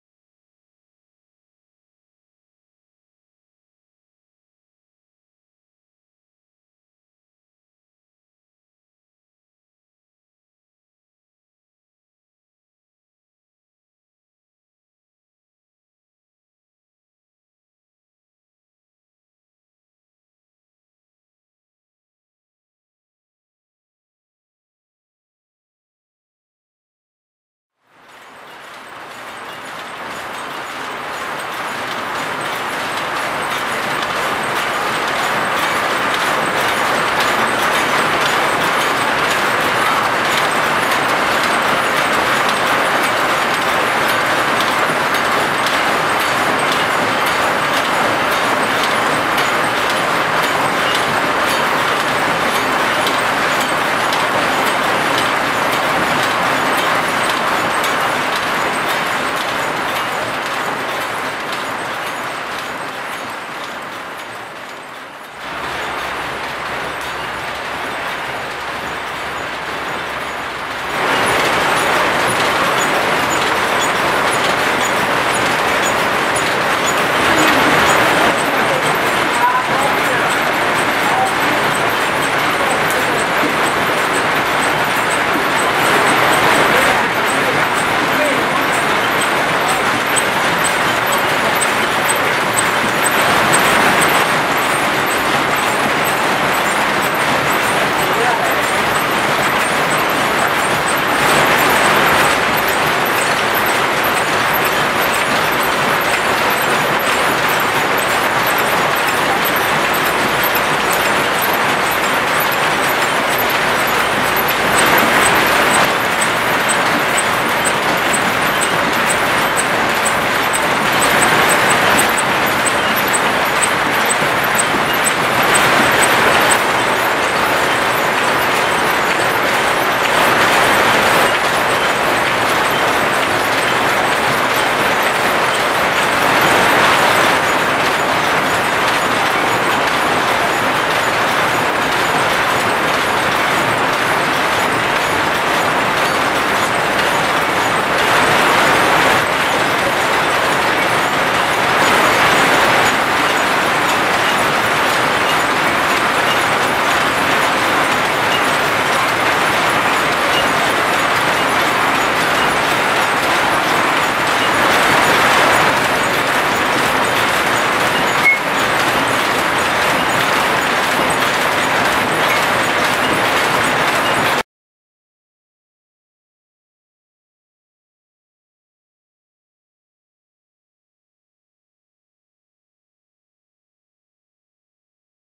making mats, China
Small machine shop outside Suzhou Industrial Park. Workshop lit by a few faintly glowing fluorescent tubes. Machines dusty and worn,  card coded looms, everything else is made by hand.